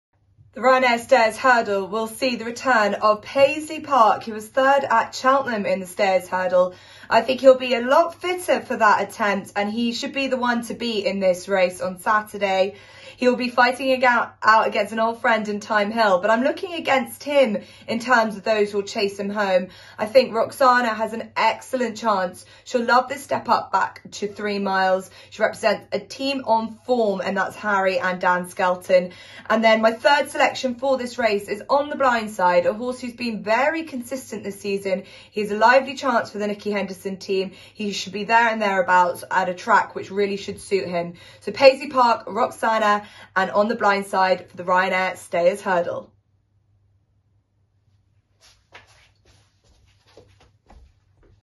Jess Stafford's Big Race Tips for Race #4 at Aintree April 10th
Jess Stafford's Big Race Tips for Race #4 at Aintree April 10th with a focus on No 8 Paisley Park.